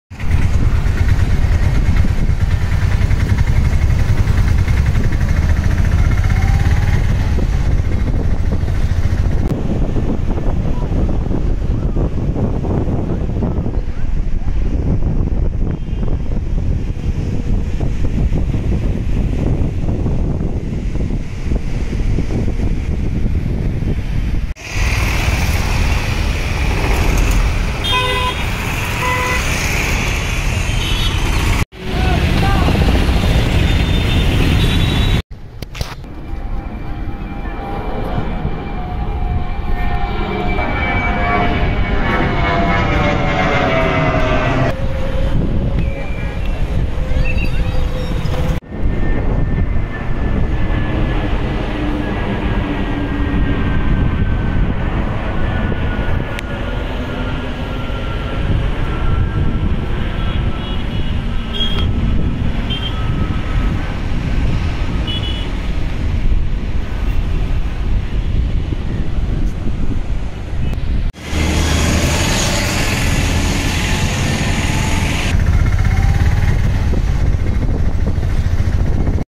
Flight Flying Videos taken from Visakhapatnam, India
Flight Flying Videos taken from Visakhapatnam, India at Day and Night. You can watch flight movements in various parts of vizag.